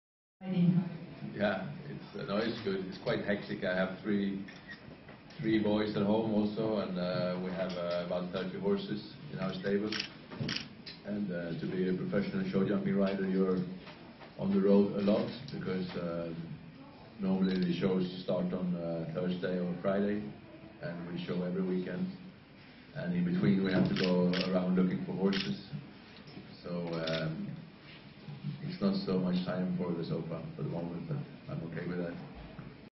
fredricson 20171019ticklen video 1508446633
Peder Fredricson kertoo urastaan HIHS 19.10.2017.